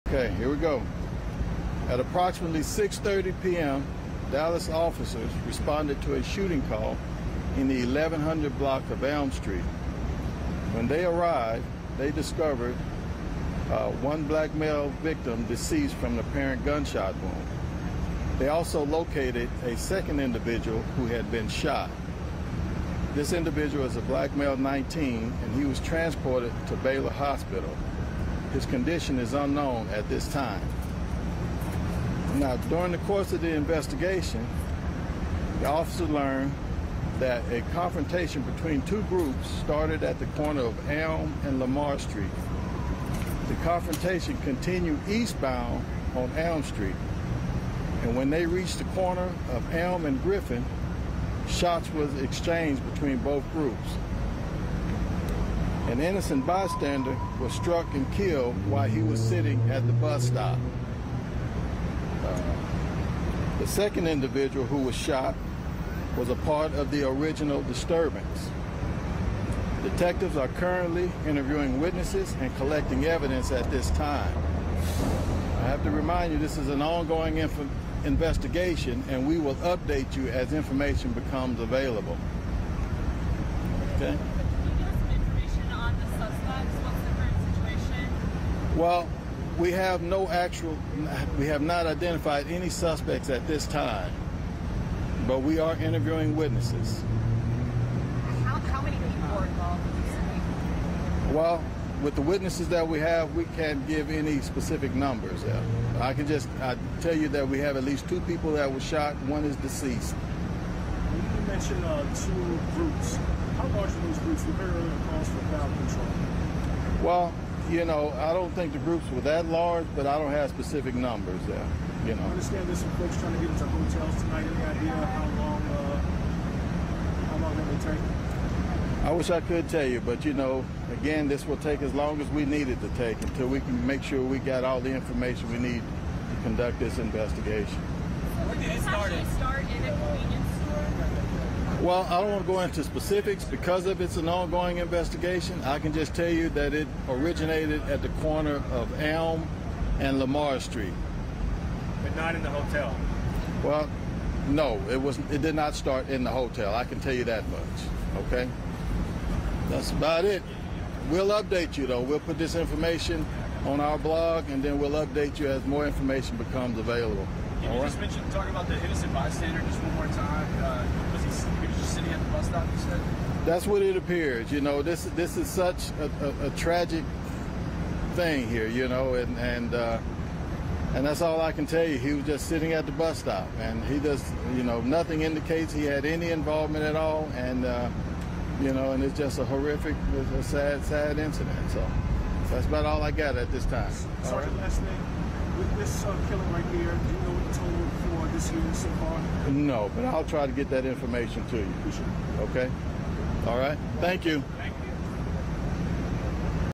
The Dallas Police Department Holds News Conference Over Multiple People Shot, One Dead Outside Dalla
Police are investigating after one man was shot dead and multiple others were injured near the Crowne Plaza Hotel in downtown Dallas Friday evening.